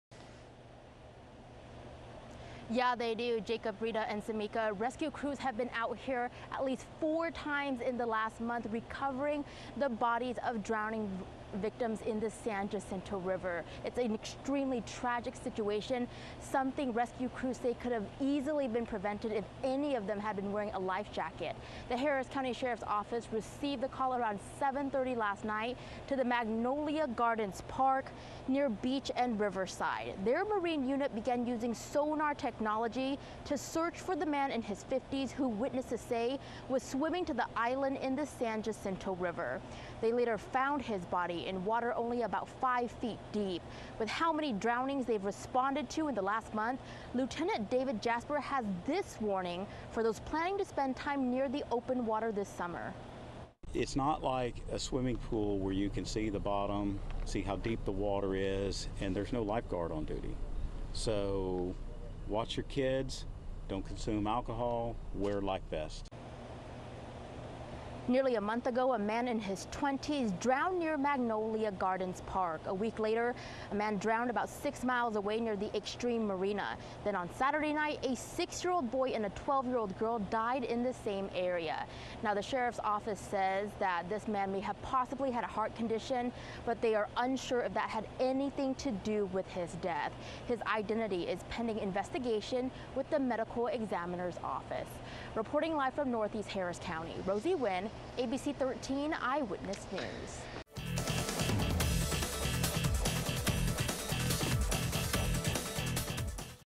Man becomes 4th drowning victim in San Jacinto River in last 30 days
Your source for breaking news and live streaming video online.